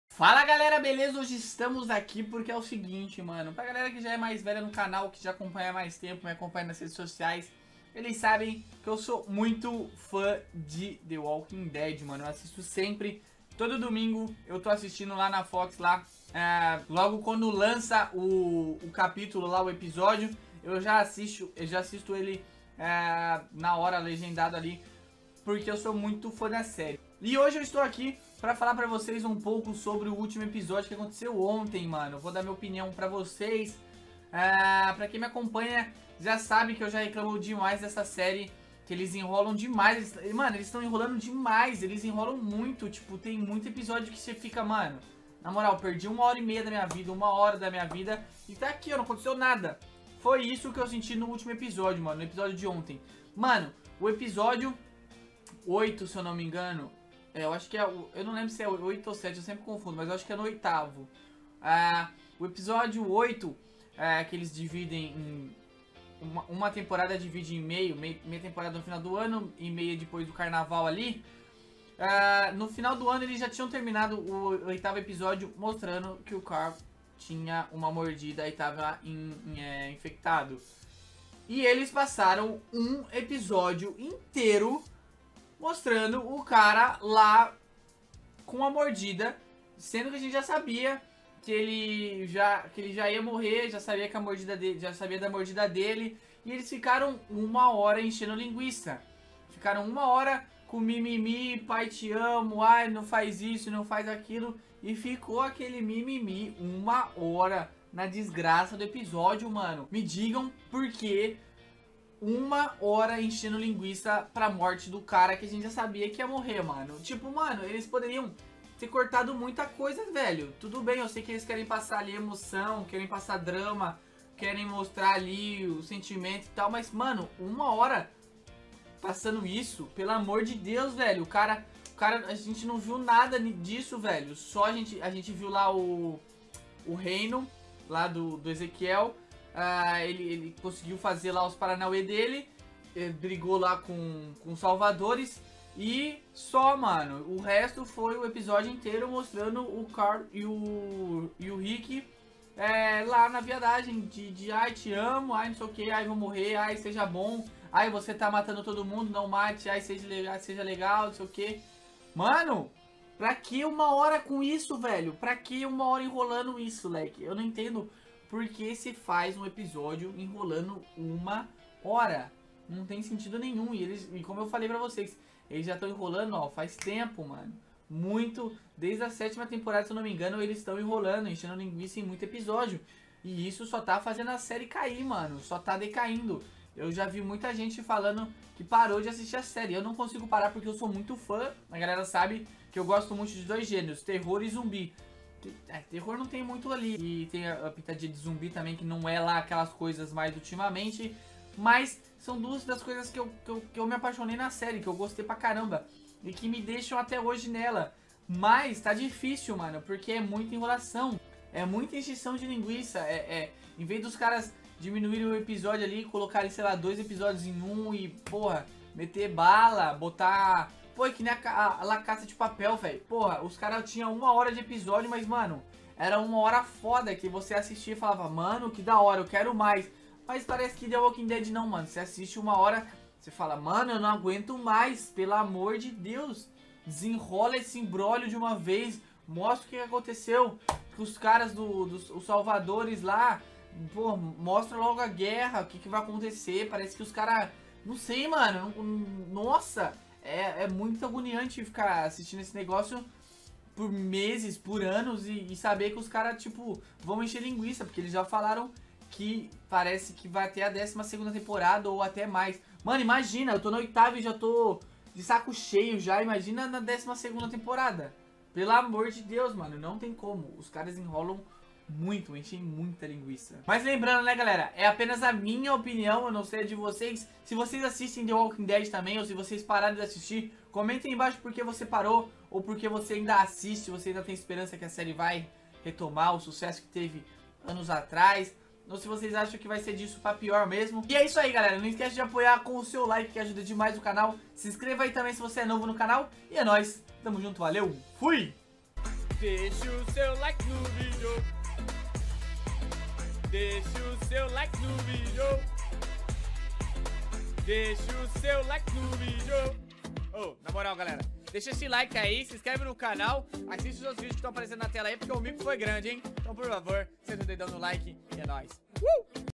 THE WALKING DEAD S8E9 - Honor ‹Critica›
minha opinião sobre a volta da série the walking dead season 8 ep 9 - honor - crítica